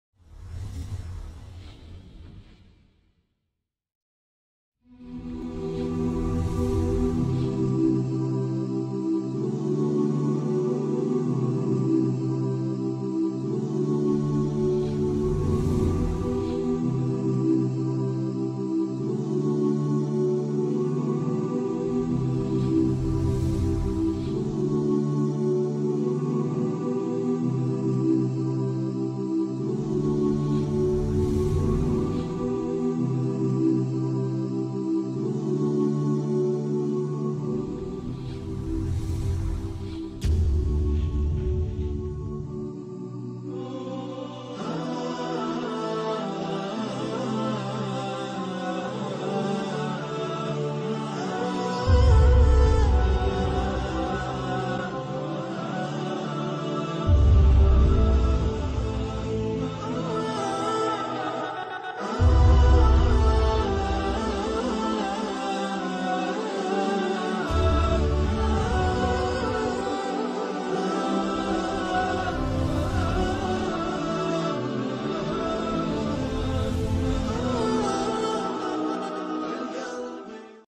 YOU: REDISCOVERED - Free Seminar to help you discover your place in the cosmos!
Why did God create us? Who are we? Where did we come from? How are we different than other creation?

Dr. Amena Syed
Shaykh Yusuf Badat
Shaykh Omar Subedar